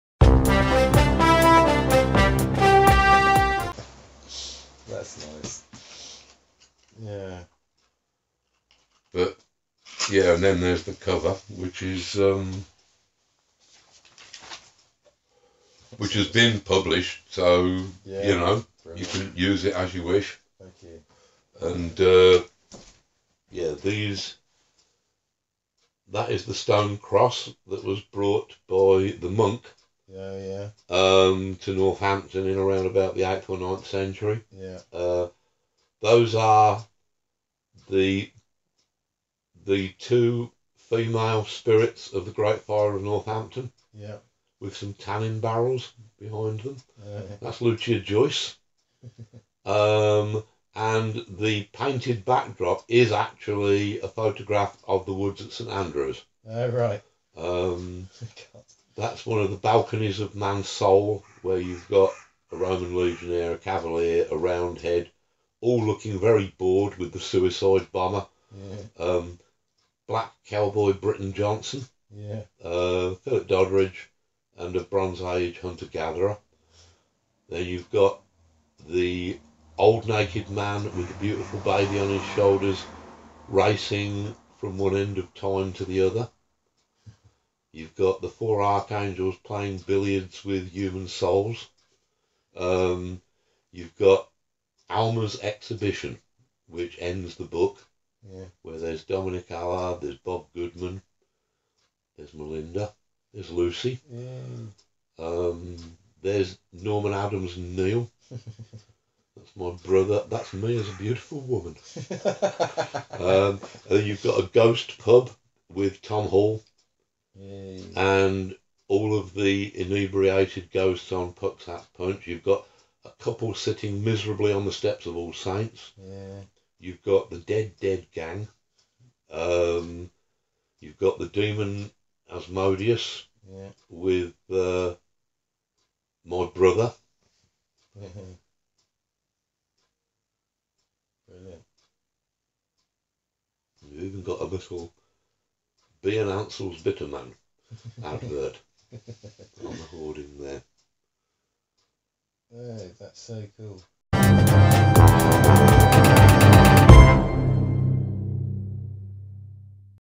Jerusalem Cover
Alan Moore decodes the cover of his epic novel Jerusalem.